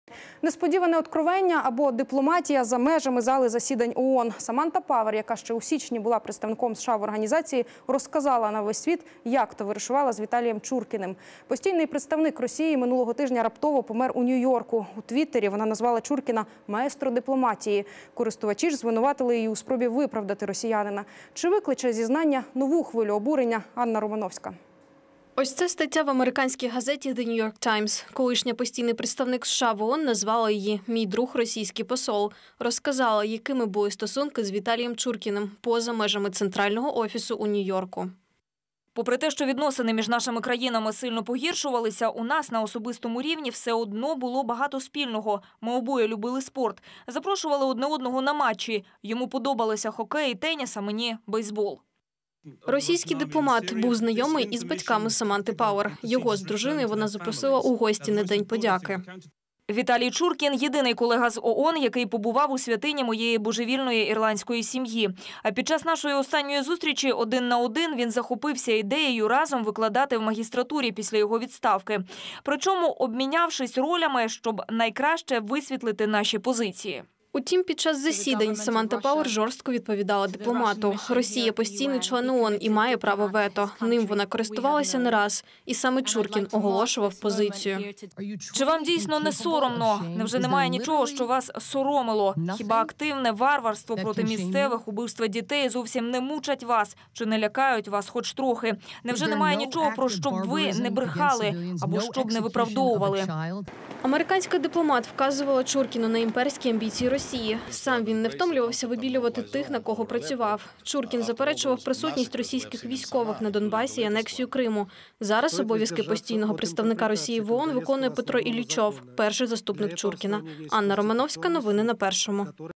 Саманта Павер розповіла, як дружила з Віталієм Чуркіним
Саманта Павер, яка ще у січні була представником США в організації – розказала на увесь світ, як товаришувала з Віталієм Чуркіним. Постійний представник Росії минулого тижня раптово помер у Нью-Йорку. У "Твіттері" вона назвала Чуркіна "маестро дипломатії" – користувачі ж звинуватили її у спроті виправдати росіянина. Чи викличе зізнання – нову хвилю обурення – Анна Романовська.